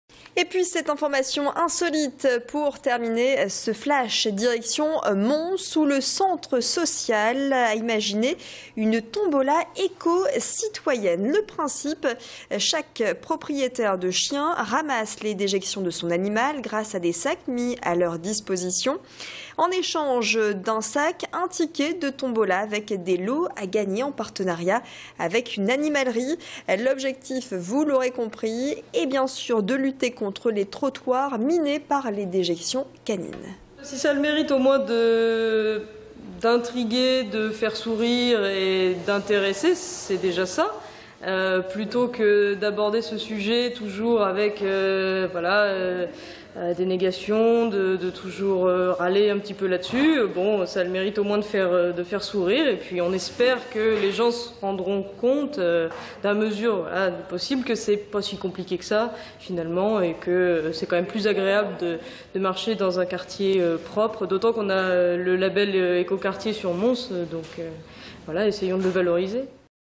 Des crottes de chien qui peuvent rapporter gros!
Gros plan sur une tombola insolite. Le principe: ramasser les déjections de son chien, en échange d'un sac, un ticket de tombola!

Sites webs/blogs, copiez cette vidéo avec le bouton "Intégrer" !